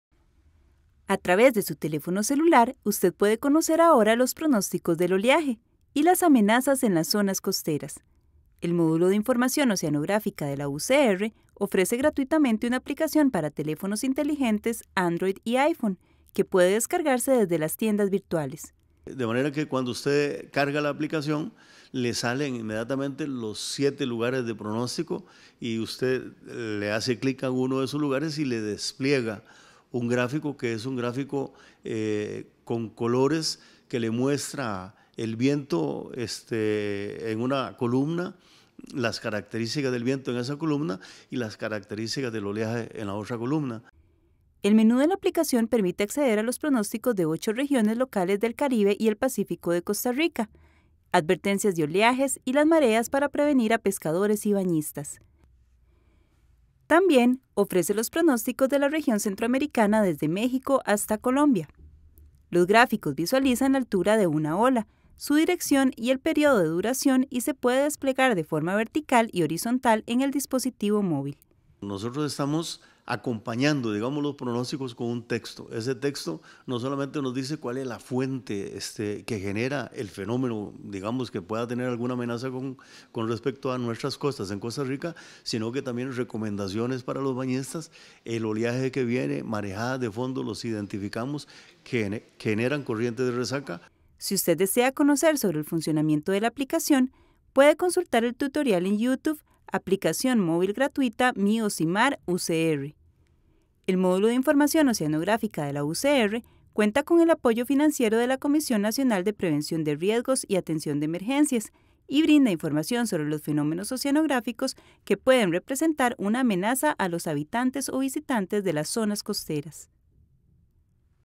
Nueva Aplicacion desarrollada por el Módulo de Información Oceanográfica de la UCR
Gráficos y notificaciones con advertencias para los pescadores y los bañistas están ahora disponibles en su celular, conozca en la siguiente nota la nueva aplicación móvil desarrollada por el Módulo de Información Oceanográfica de la UCR para teléfonos inteligentes.